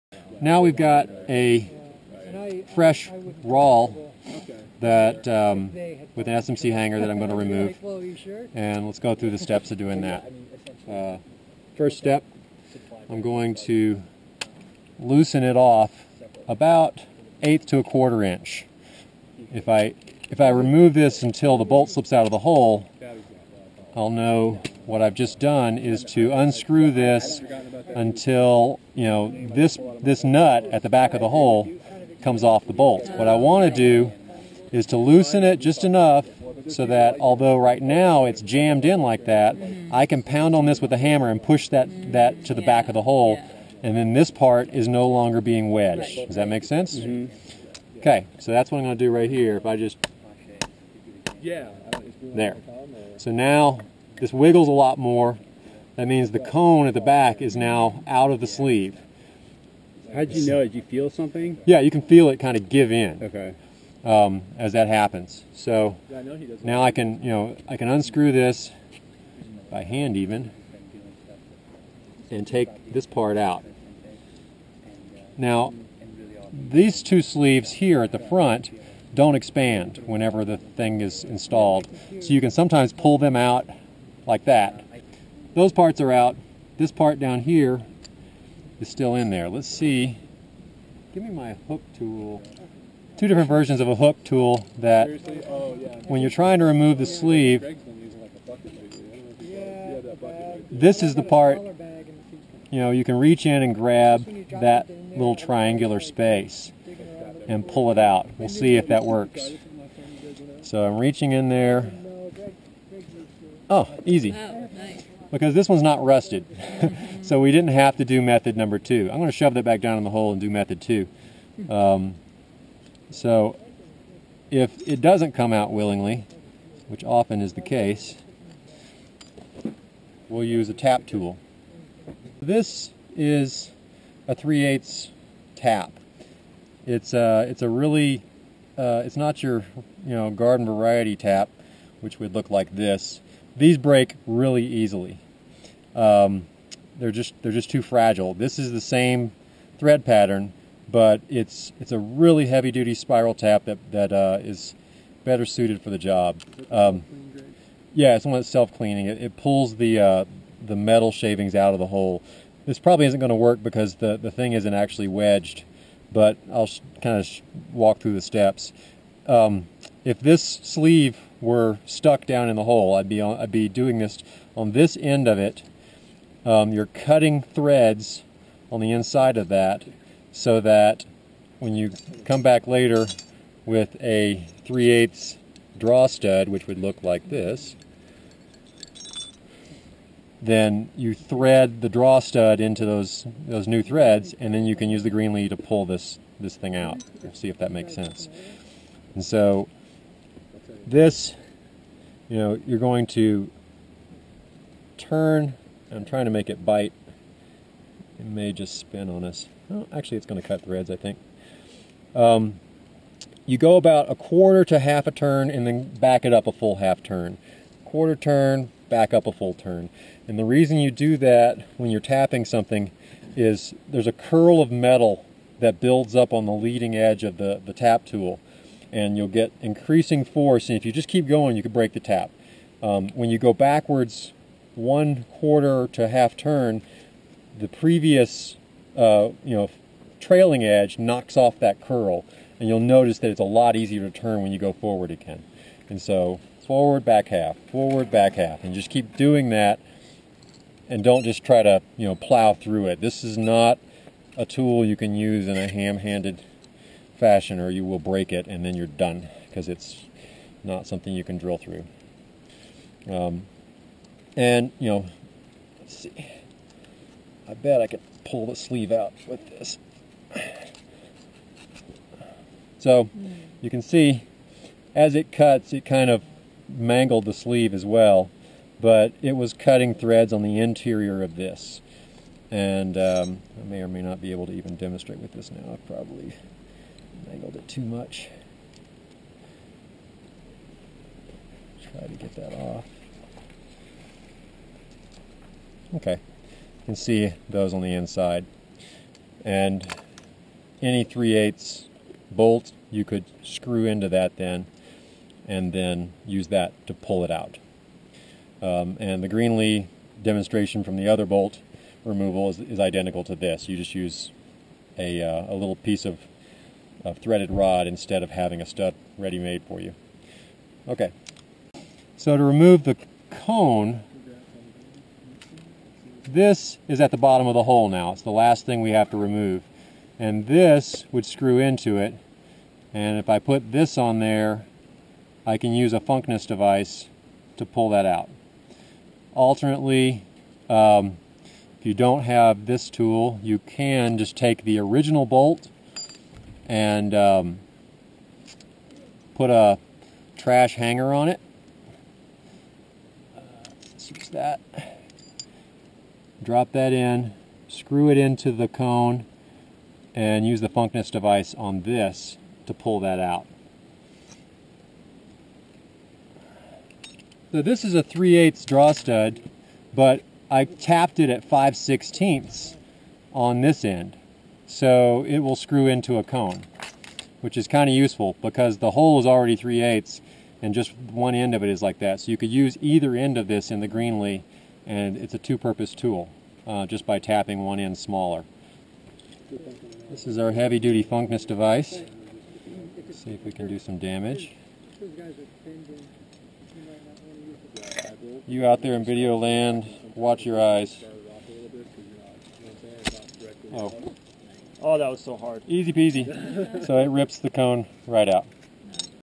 Sustainable re-bolting: Rawl/ 5-piece removal
How to remove a 5-piece bolt: push the cone to the back of the hole, hook the extension sleeves if possible, then use a tap to cut threads on the interior surface of the sleeve(s) that remain stuck. Pull out the sleeves using A Greenlee punch driver, an eyebolt and funkness device, or some other puller contraption. Extract the cone by similar means using either the original bolt or a draw stud.